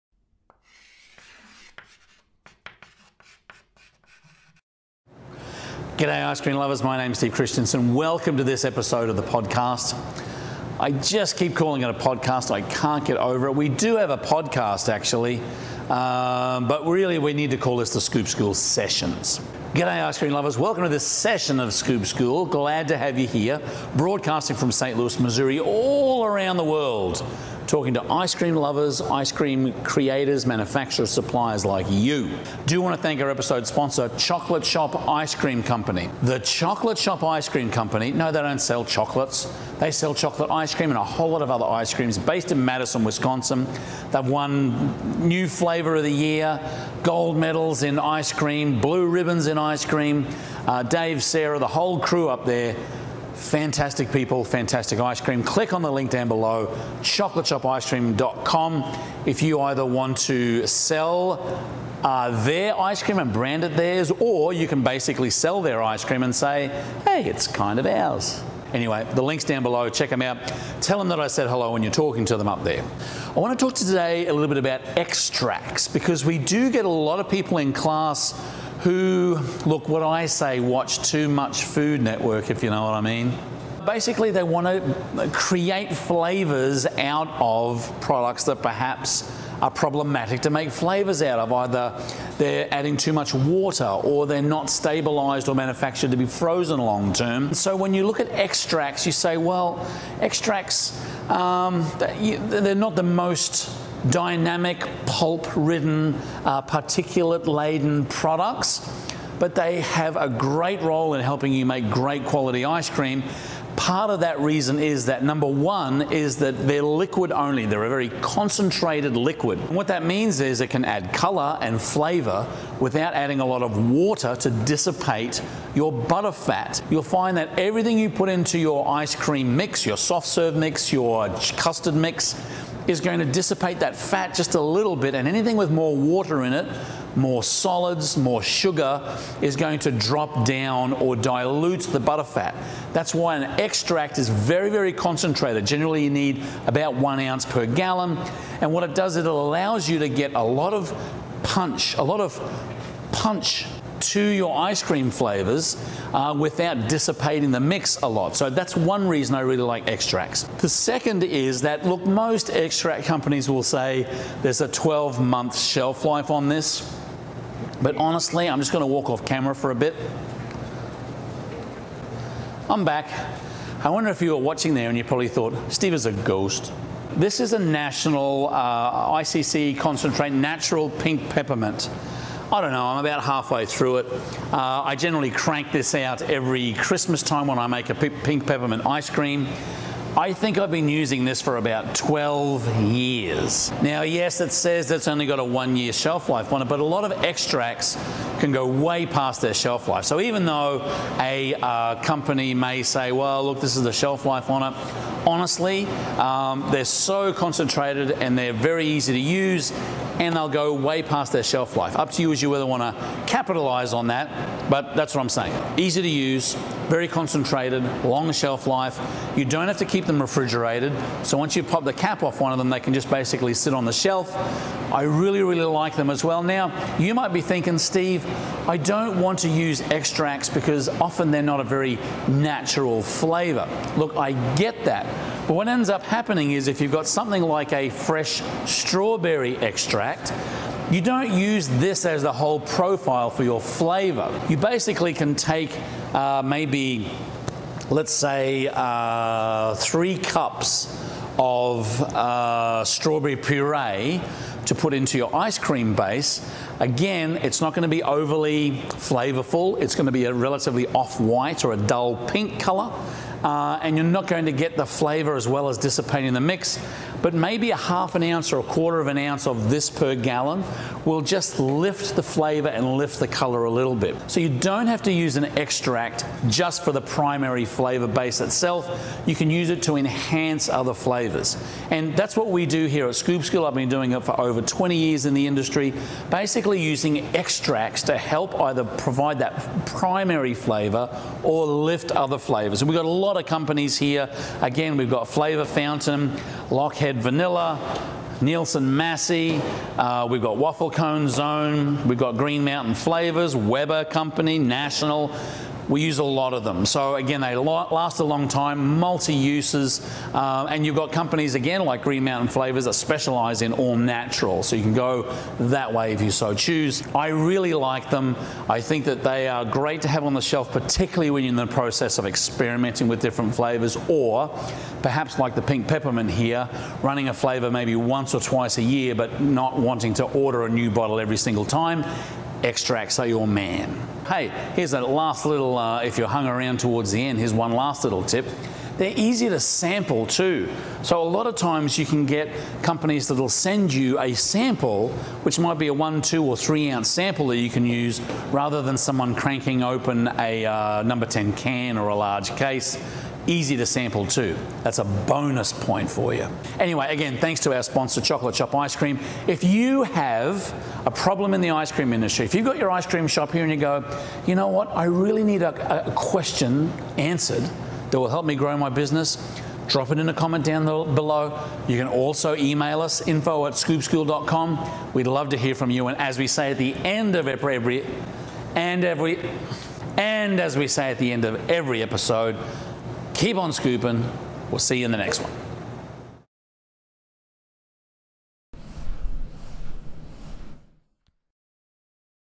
Should you use Extracts in your Ice Cream?
Steve Christensen, The Ice Cream Bloke and Self-Appointed Headmaster of Scoop School, talks in this episode about extracts and how to use them in your ice cream.